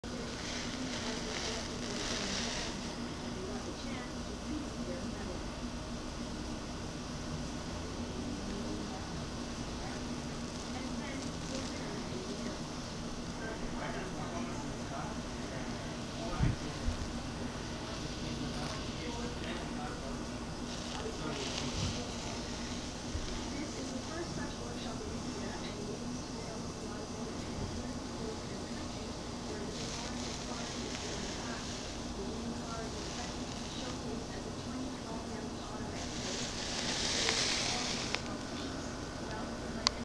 Boa Constrictor on the floor exploring
My 6.5 foot boa close up exploring the hallway...